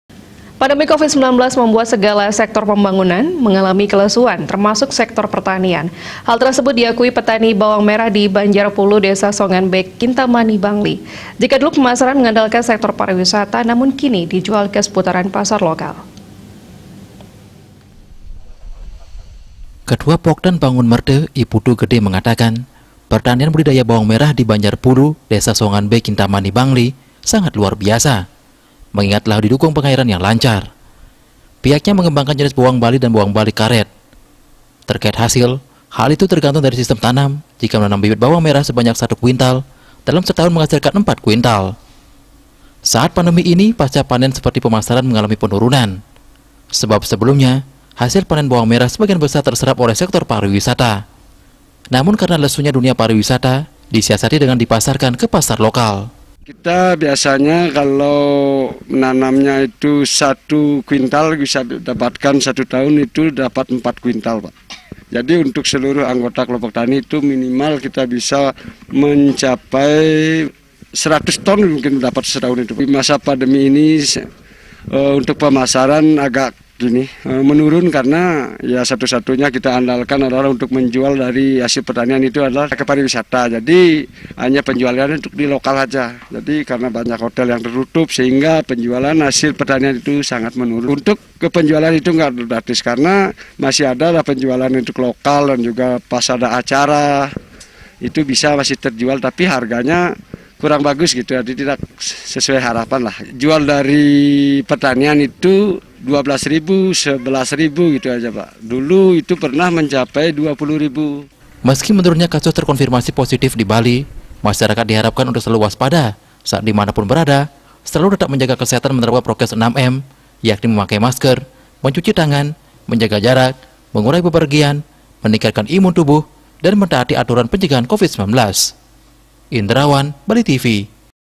PENJUALAN HASIL PANEN BAWANG MERAH MENGARAH PASAR LOKAL
PANDEMI COVID-19 MEMBUAT SEGALA SEKTOR PEMBANGUNAN, MENGALAMI KELESUAN, TERMASUK SEKTOR PERTANIAN. HAL TERSEBUT DIAKUI PETANI BAWANG MERAH DI BANJAR PULU, DESA SONGAN B KINTAMANI BANGLI. JIKA DULU PEMASARAN MENGANDALKAN SEKTOR PARIWISATA,  NAMUN KINI DIJUAL KE SEPUTARAN PASAR LOKAL.